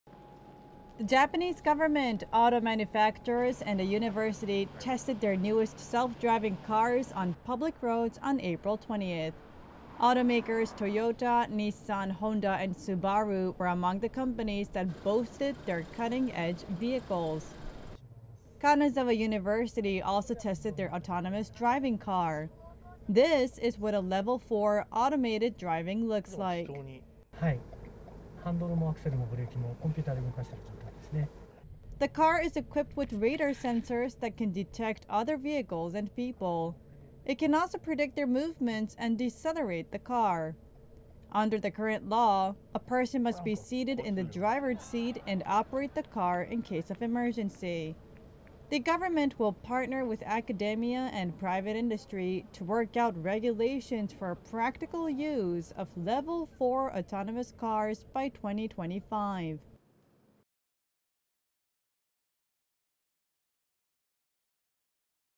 Self driving cars run on public roads
The Japanese government, auto manufacturers and a university test self-driving cars on public roads in Tokyo.